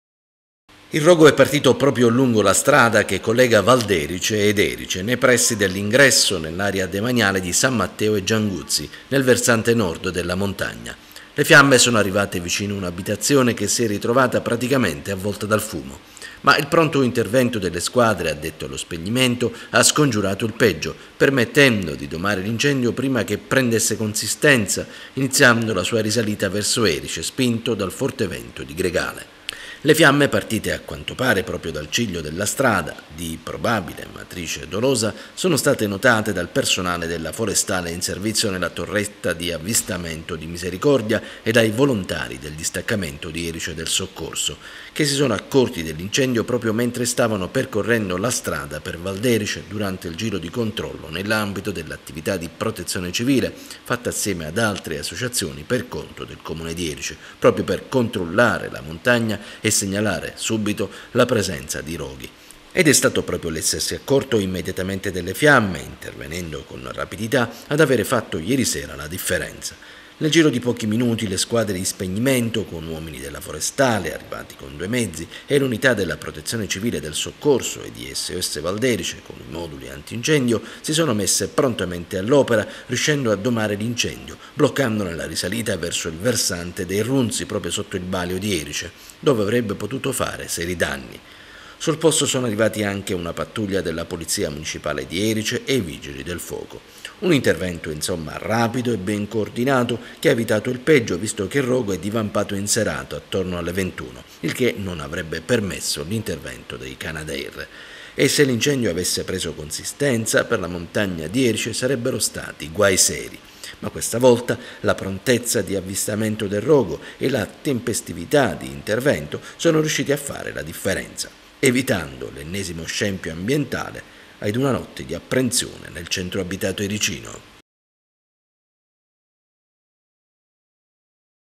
Incendio della montagna di Erice
Incendio ieri sera nella montagna di Erice. Il pronto intervento delle squadra addette allo spegnimento ha evitato il peggio.